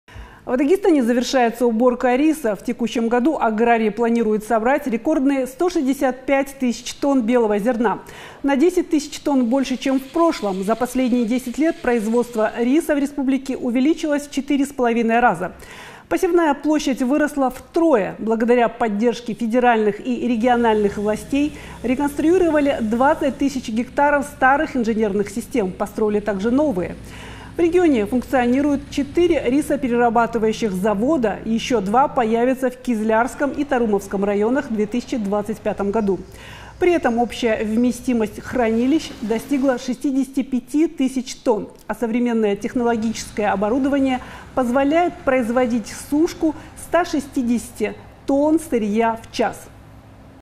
В Дагестане завершается уборка риса
В текущем году аграрии планируют собрать рекордные 165 тыс. тонн риса — на 10 тыс. больше, чем в прошлом. За последние десять лет производство риса в республике увеличилось в 4,5 раза, посевная площадь выросла втрое.